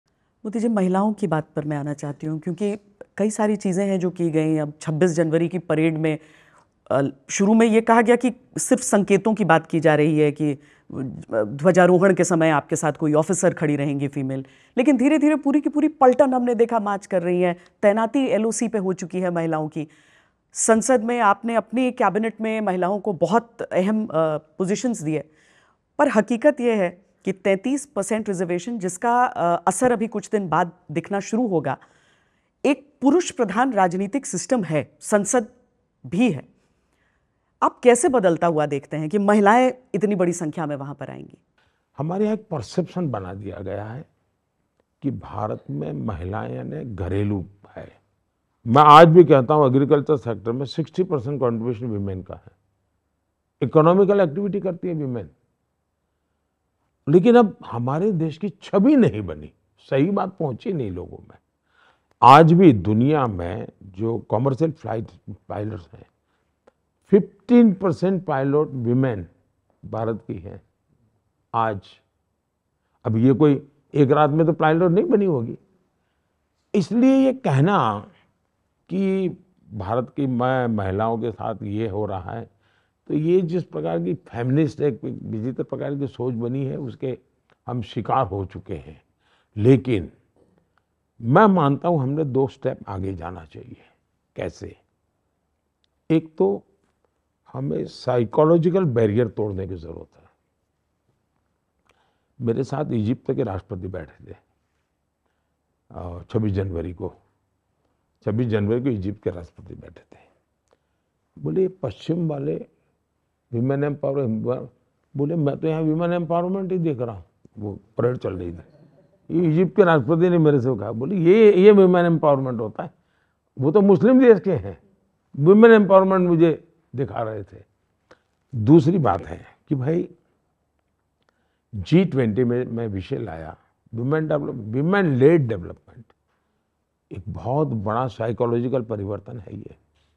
‘Women-led development’ is a big psychological change: PM Modi
Prime Minister Narendra Modi, during an interview with Aaj Tak and India Today, spoke on a range of topics. Speaking to the journalists, PM Modi shed light on why elections should not be a 5-year agenda. Further, he also said that questions should first be asked to the family who misruled the nation and tampered with the Constitution.